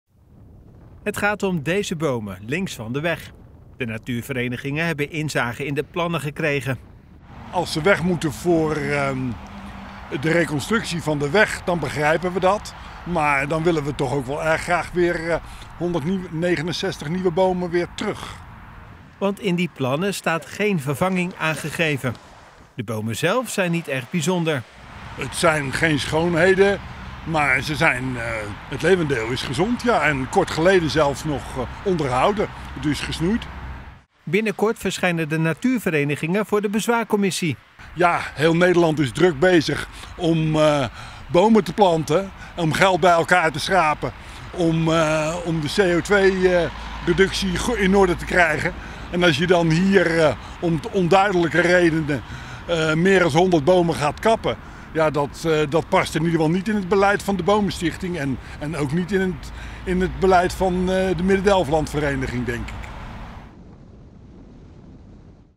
Bezwaar tegen het kappen van 169 bomen
De Bomenstichting en de werkgroep Bomen van de Midden-Delfland Vereniging hebben bezwaar gemaakt tegen de kapvergunning van 169 bomen langs de N223 bij 't Woudt.